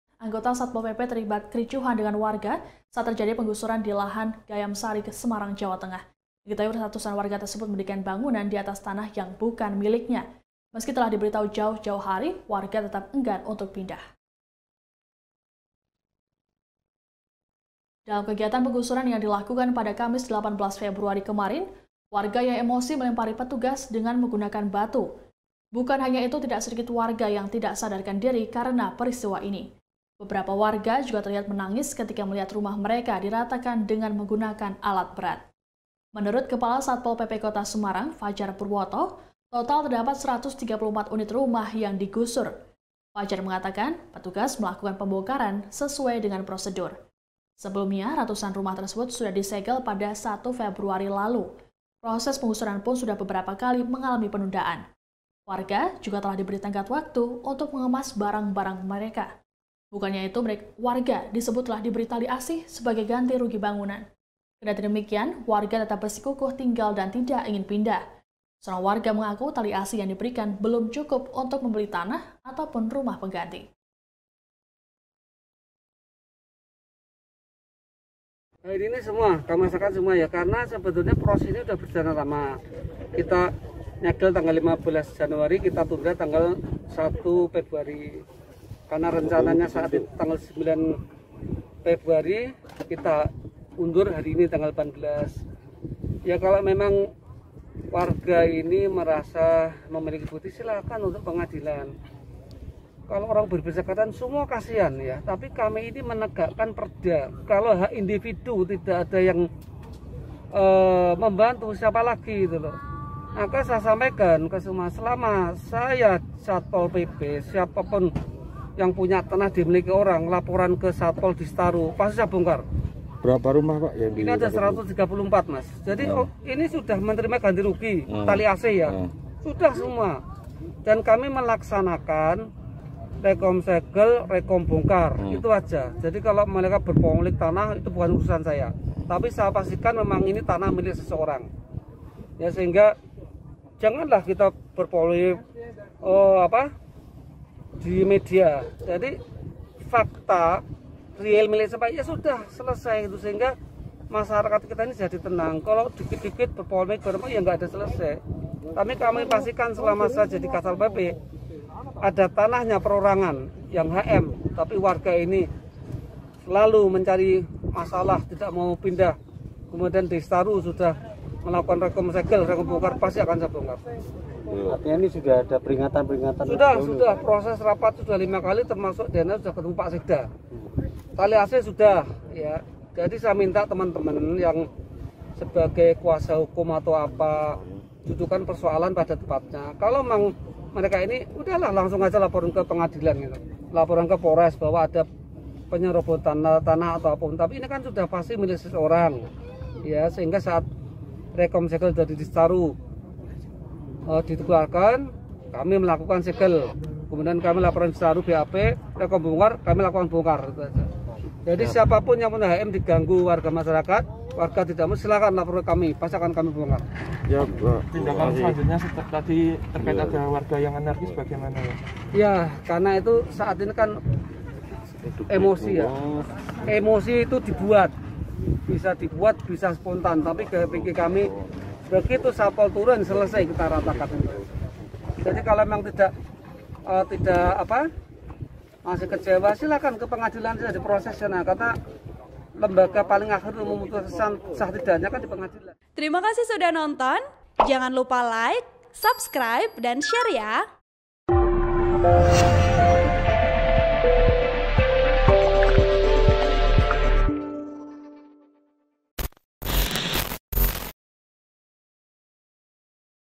Tak Terima Rumahnya Digusur, Ratusan Warga di Gayamsari Lempari Petugas Satpol PP dengan Batu
TRIBUN-VIDEO.COM - Anggota Satpol PP terlibat kericuhan dengan warga saat terjadi penggusuran lahan di Gayamsari, Semarang, Jawa Tengah.

Diketahui ratusan warga tersbeut mendirikan bangunan di atas tanah yang bukan miliknya.

Meski telah diberitahu dari jauh-jauh hari, warga tetap enggan untuk pindah.